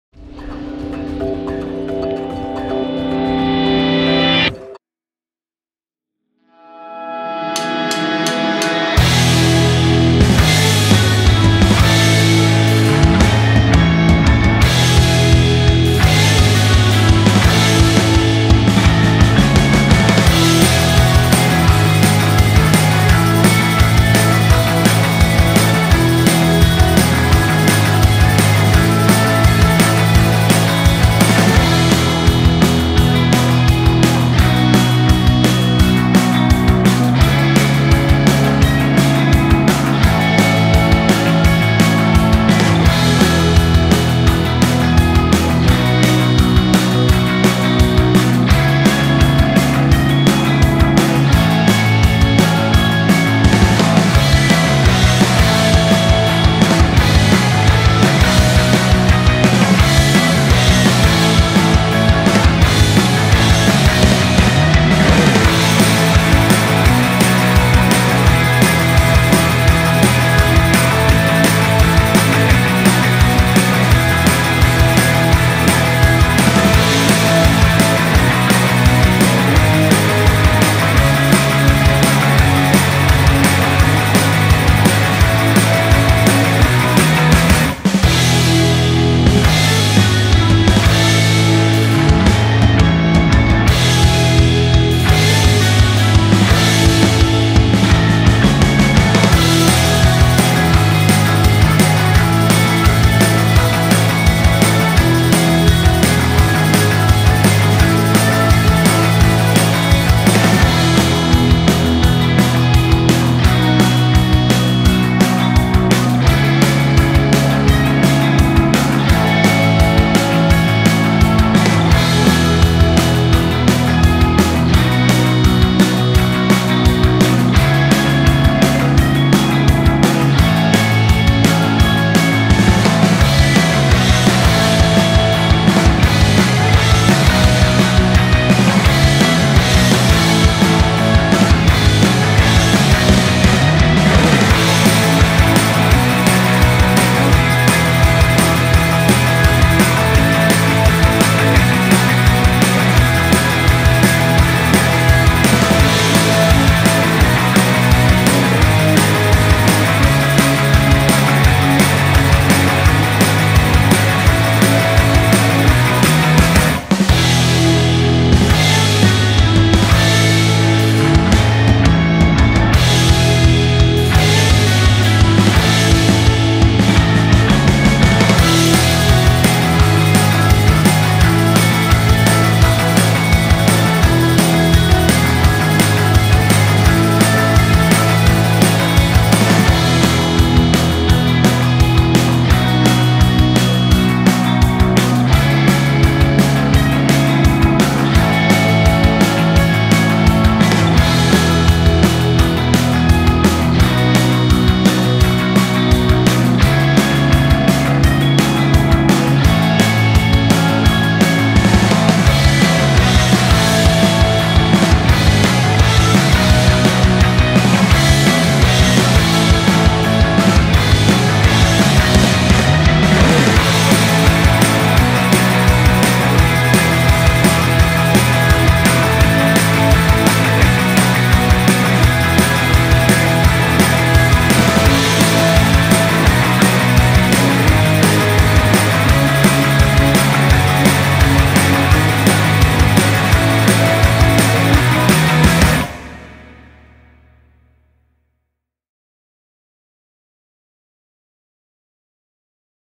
Shoegaze/Dream Pop Backing Track in E major
Trap/Chill/Ambient/House etc... with Guitar track

KEY=E major
BPM=170